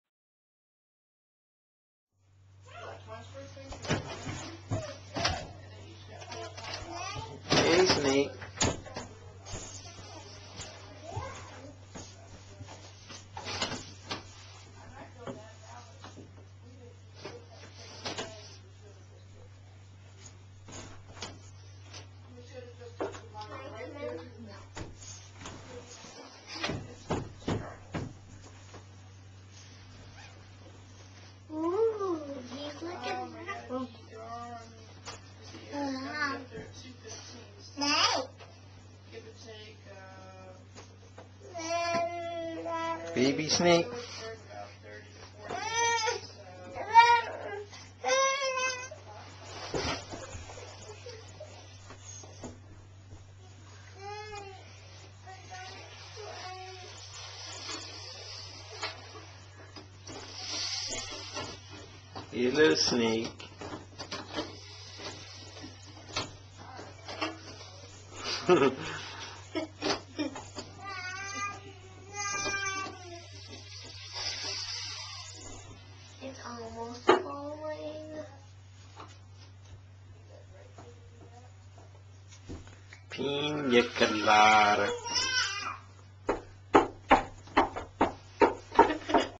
Tantilla Planiceps - "The smallest venomous snake in North America"
This small snake was at the gas station when I was gassing up the van tonight.